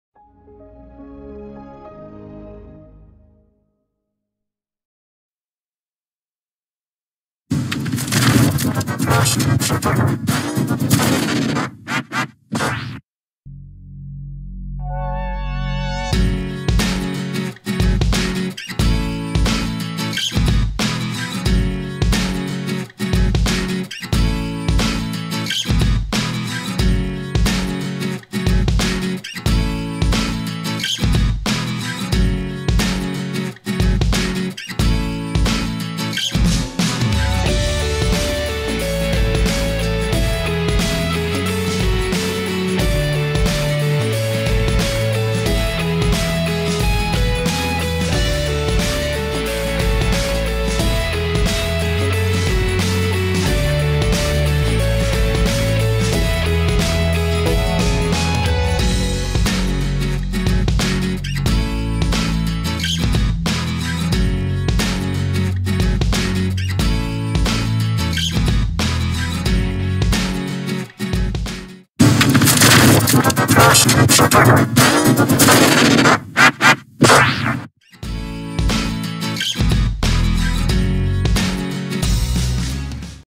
How To Make G-Major 5918 On Vegas Pro
Made on ‎Thursday, ‎October ‎21, ‎2021, ‏‎10:43:30 pm to 10:43:55 pm

Let's learn how to make G-Major 5918 made first on Vegas Pro.

How To Make G-Major 5918:

(VideoFX)

1st: TV Simulator - Line Sync 0.7500

2nd: Mirror - Relfect Left

3rd: Channel Blend - RGB to BGR

4th: HSL Adjust - Hue 0.400

Add To Hue:  0.400
Saturation:  1.000
Luminance:   1.000

(AudioFX)

elastique
Pro

-10
-7
-2
+1
+3
+7
+10

Effect by Carlos Virgílio

(NEW EFFECT) Klasky Csupo in G Major 5918
1,457 views